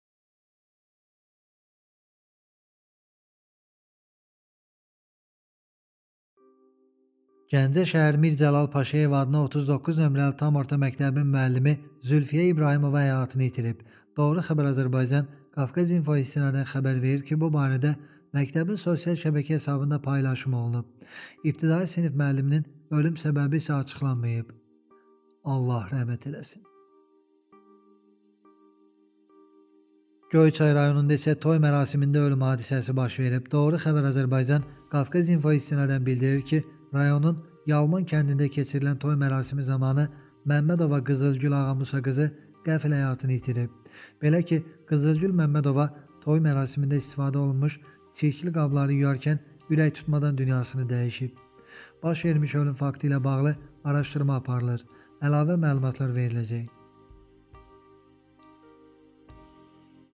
Gəncədə müəllim, Göyçayda isə toyda xadimə vəfat etdi - Doğru Xəbər Az
Gəncədə müəllim, Göyçayda isə xadimə vəfat etdi - Doğru Xəbər Az